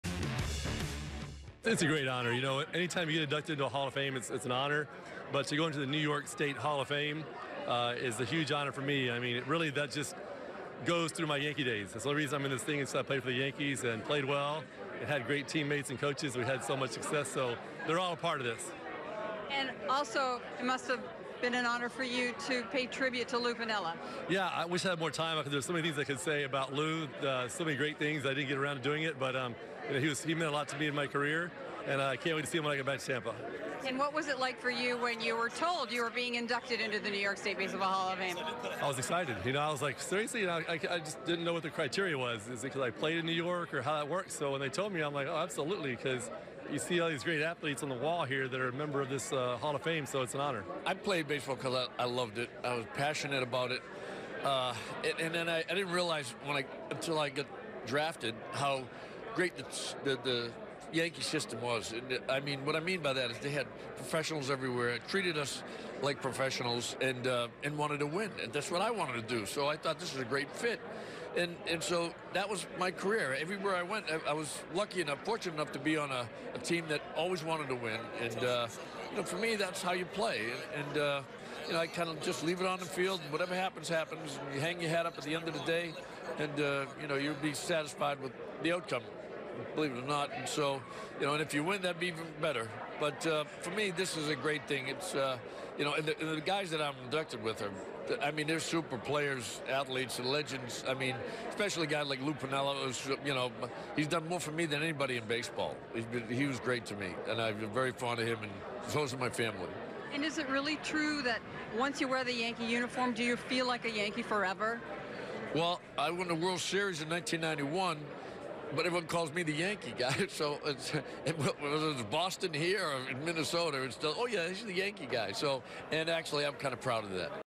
Tino Martinez and Lou Piniella NYS HOF induction interview
Tino Martinez and Lou Piniella were inducted into ther NYS Baseball HOF, last night Nancy Newman caught up with them to discuss the honor.